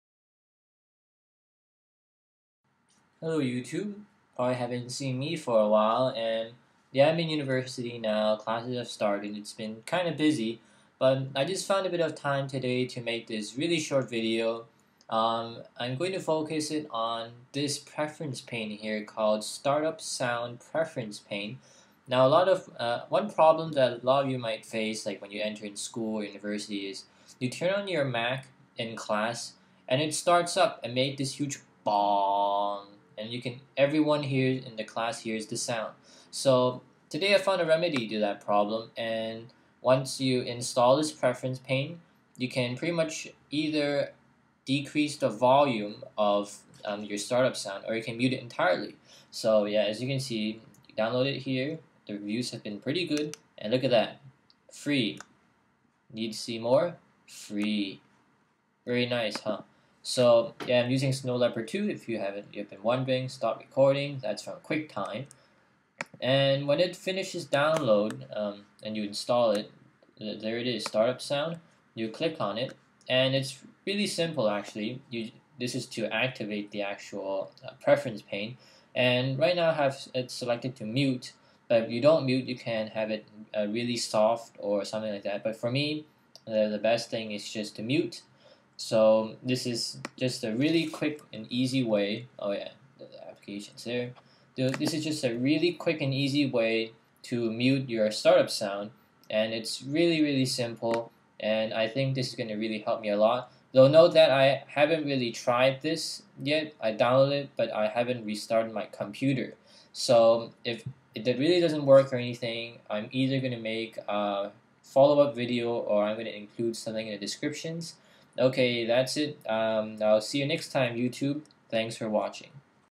Mute Mac Startup Sound
Hello, I come with another video!  Basically using the preference pane you can mute the startup sound when your mac boots up, pretty handy especially if you're in the library/in class etc.  I have yet to test this out, and I'll update this description when I find out how it goes.  This doesn't not affect the master volume control, which is awesome.